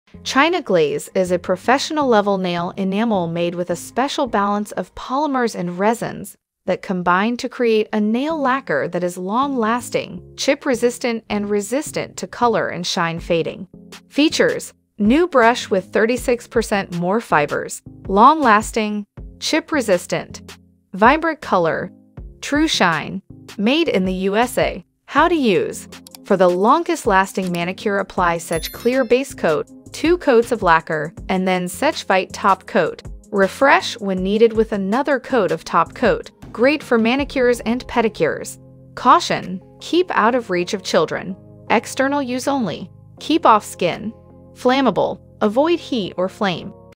China Glaze Nail Polish Which Every Girl Must Have in Her Purse.
China Glaze is a professional-level nail enamel made with a special balance of polymers and resins that combine to create a nail lacquer that is long-lasting, chip-resistant, and resistant to color and shines fading.